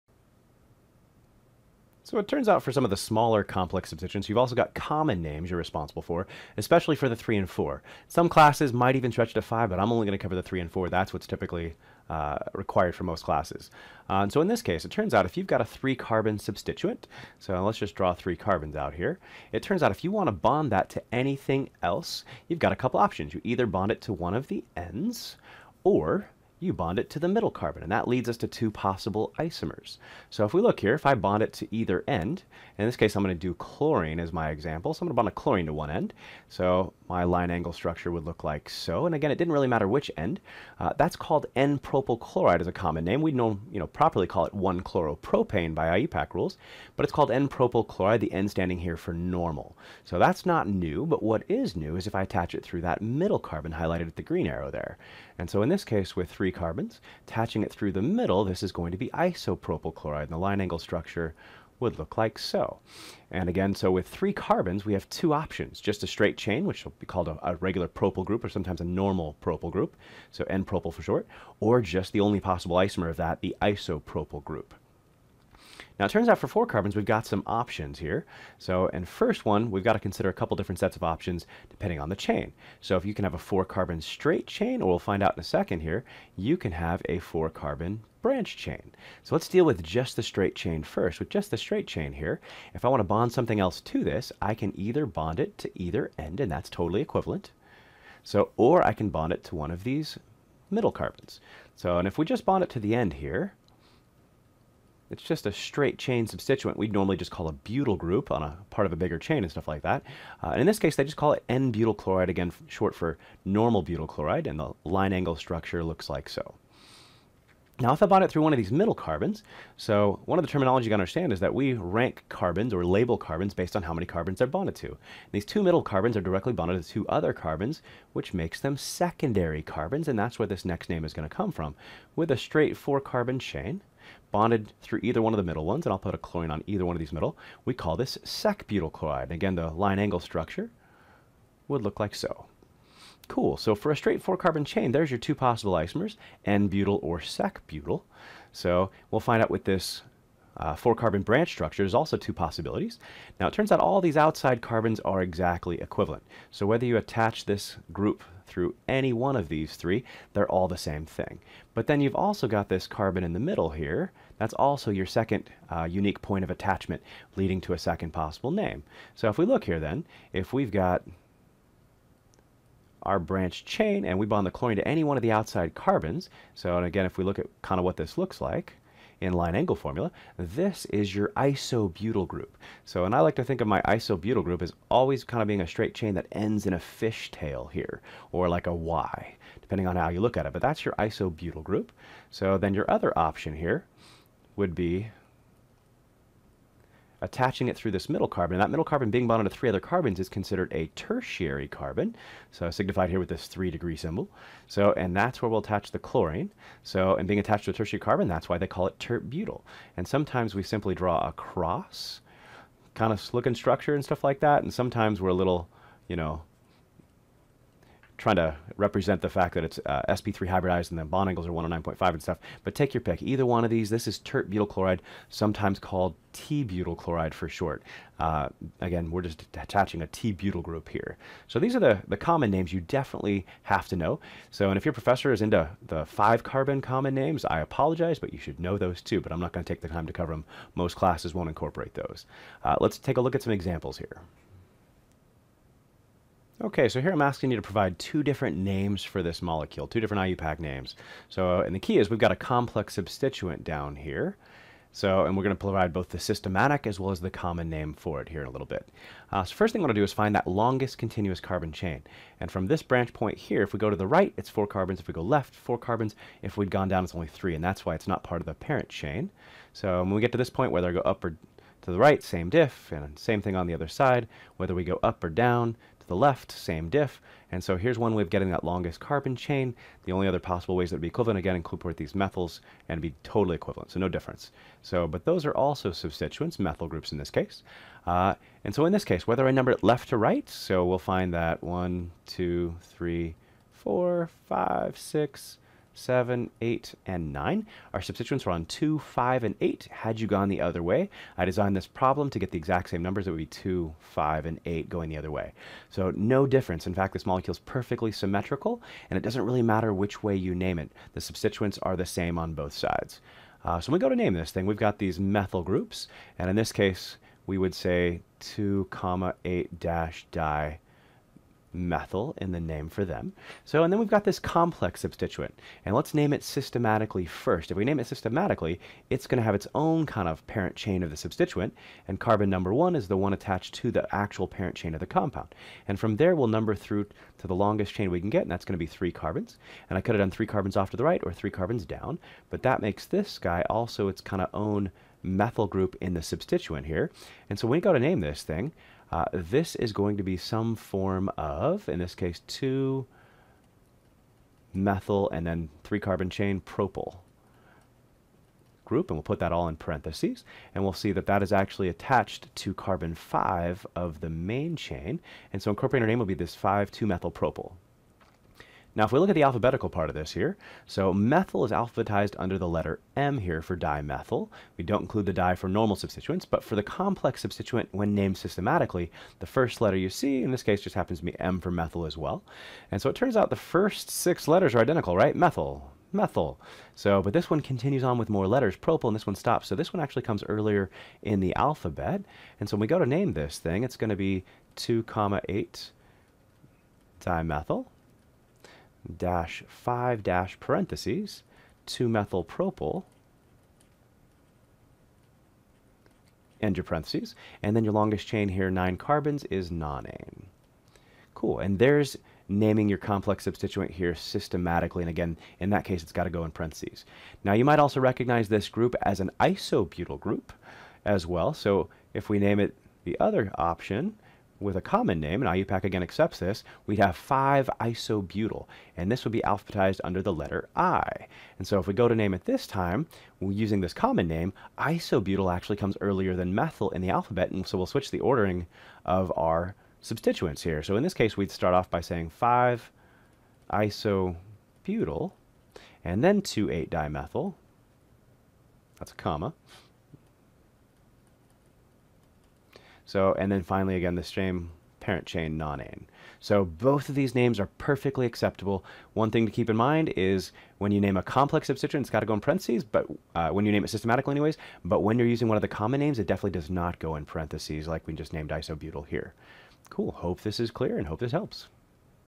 4.1d Common Names of Complex Substituents
Chad breaks down the common names for Complex Substituents including isopropyl, isobutyl, sec-butyl, and tert-butyl groups.

Happy Studying!